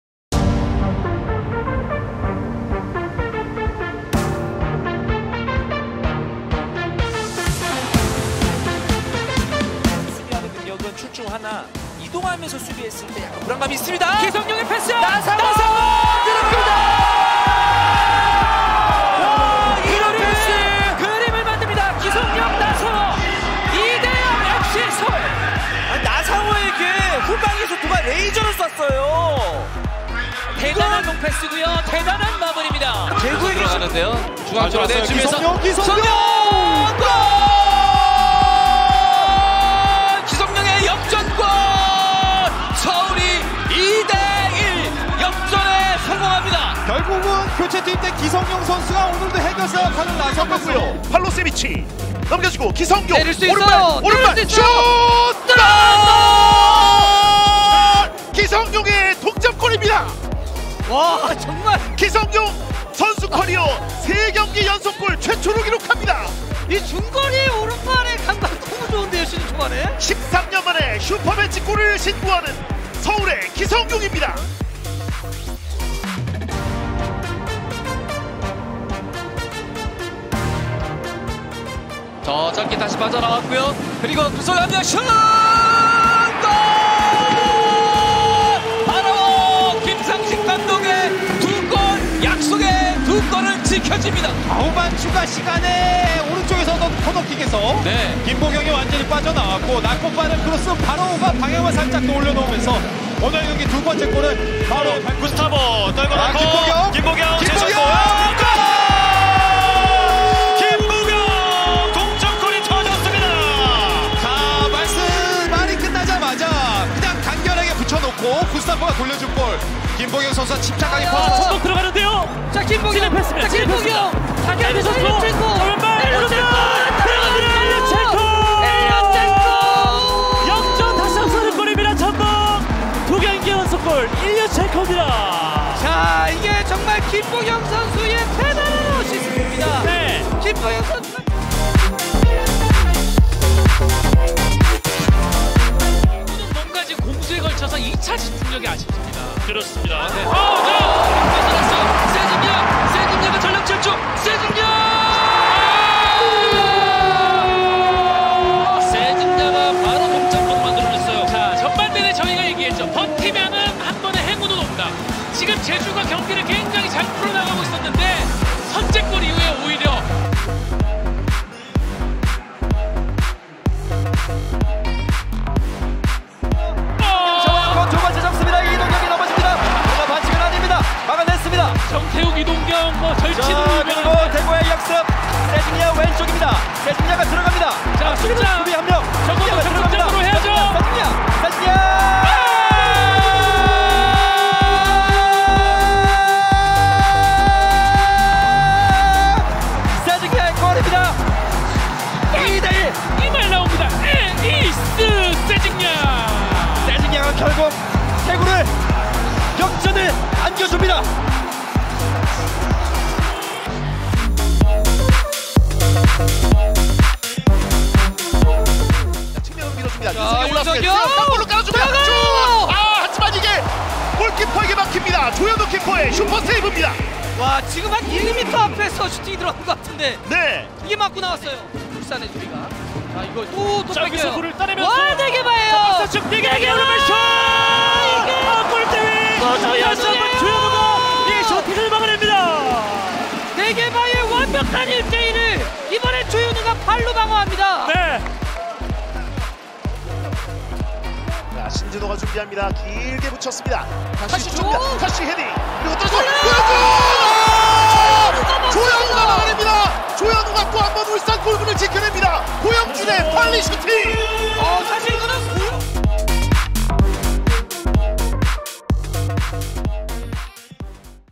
EA 이달의 선수상 3월 후보 | EA Player Of The Month March Nominees
[POTM] "3월 최고의 K리거는 누구?"
EA SPORTS 이달의 선수상 3월의 후보자
EA SPORTS Player Of The Month MARCH Nominees
서울
전북
대구
울산
🗳투표는 4월 8일 오전10시부터 11일 23시 59분까지